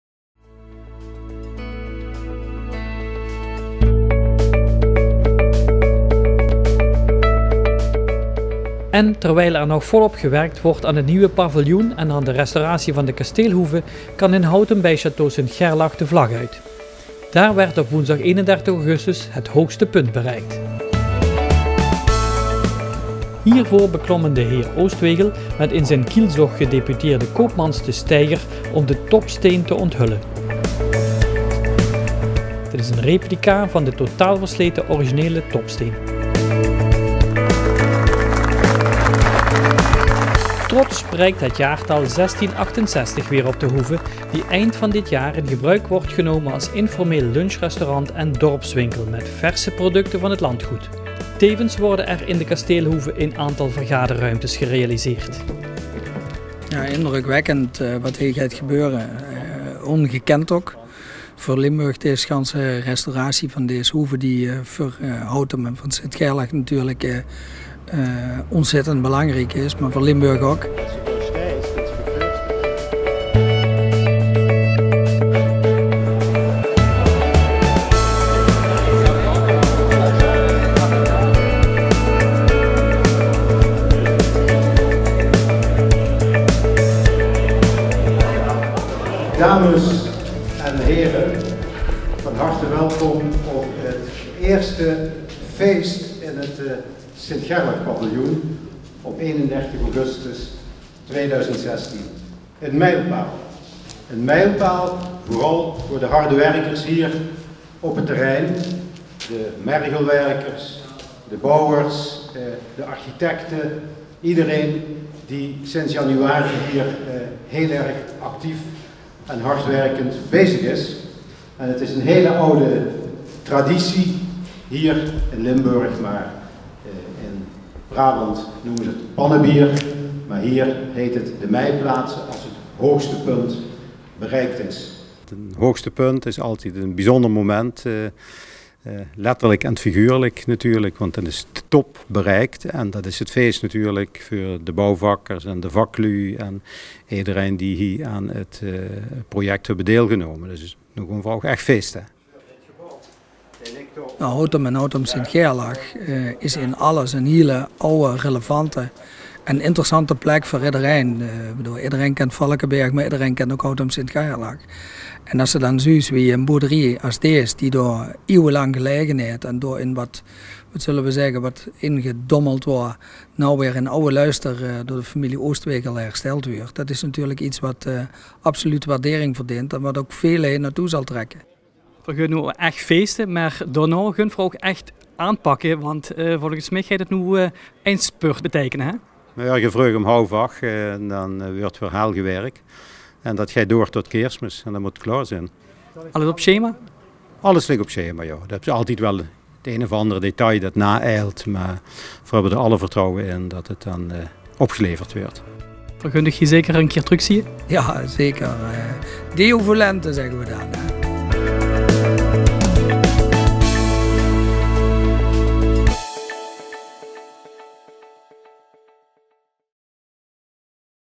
Kasteelhoeve: hoogste punt | Château St. Gerlach | Oostwegel Collection
Vanaf de bouwplaats, hoogste punt 31 augustus 2016. Bron: Henk Kroese. → volg alle ontwikkelingen op de voet via ons blog.